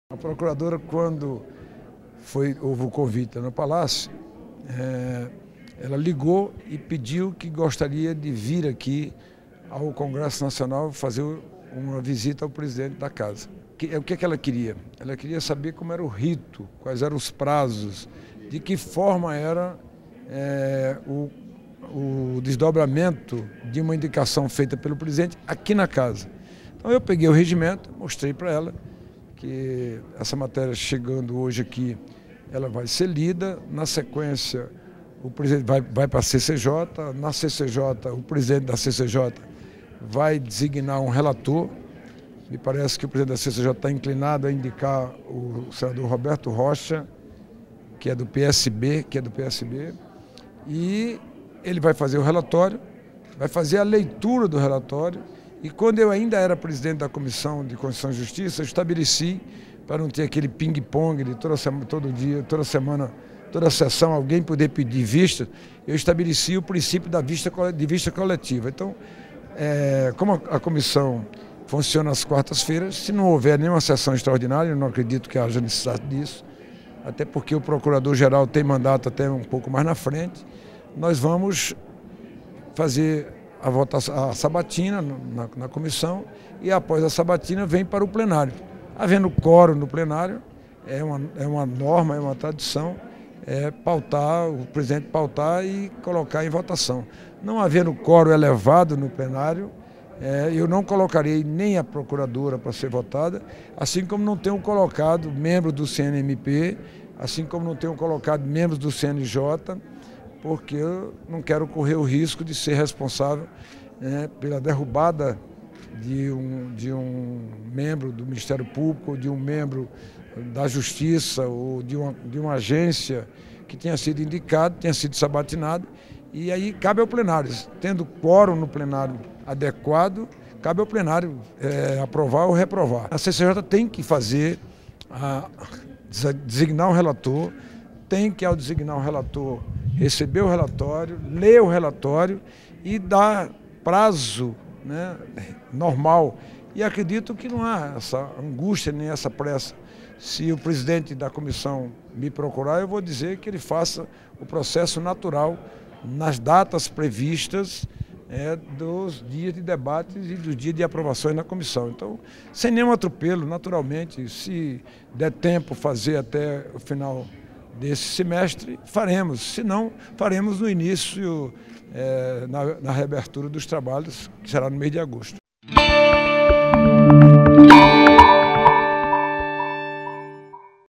Eunício detalha procedimento para aprovação de nova procuradora-geral
O presidente do Senado Federal, Eunício Oliveira, explicou nesta quinta-feira (29) o procedimento para a confirmação, na Casa, do nome de Raquel Dodge para a Procuradoria Geral da República. Dodge se reunira com Eunício na véspera. "Essa matéria vai ser lida, vai para a CCJ, que vai designar um relator. Vamos fazer a sabatina e depois vem para o Plenário. Tendo quórum adequado, cabe ao Plenário aprovar ou reprovar. Sem nenhum atropelo, se der tempo fazer até o final deste semestre, faremos. Senão, na reabertura dos trabalhos, em agosto", explicou o presidente do Senado.